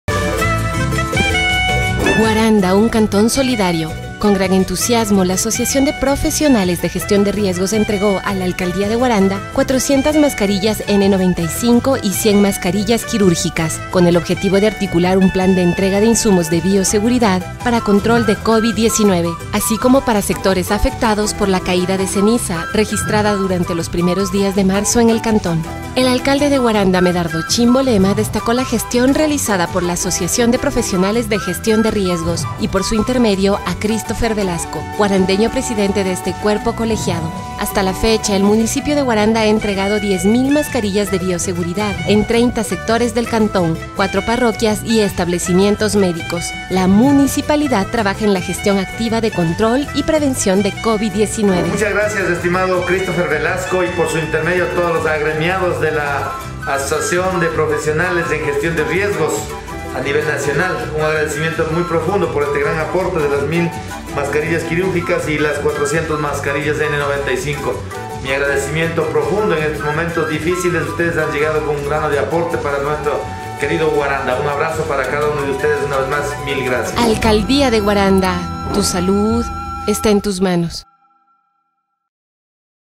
Guaranda, un Cantón Solidario
Con gran entusiasmo, la Asociación de Profesionales de Gestión de Riesgos entregó, a la Alcaldía de Guaranda, 400 mascarillas N 95 y 1000 mascarillas quirúrgicas, con el objetivo de articular un plan de entrega de insumos de bioseguridad para control de Covid 19, así como para los sectores afectados por la caída de ceniza, registrada durante los primeros días de marzo en el cantón.
El alcalde de Guaranda, Medardo Chimbolema destacó la gestión realizada por la Asociación de Profesionales de Gestión de Riesgos y por su intermedio a Cristopher Velasco, Guarandeño presidente de este cuerpo colegiado.
Hasta la fecha, el Municipio de Guaranda ha entregado 10.000 mascarillas de bioseguridad en 30 sectores del cantón, 4 parroquias y establecimientos médicos.
La Municipalidad trabaja en la gestión activa de control y prevención y control de Covid 19 en el cantón.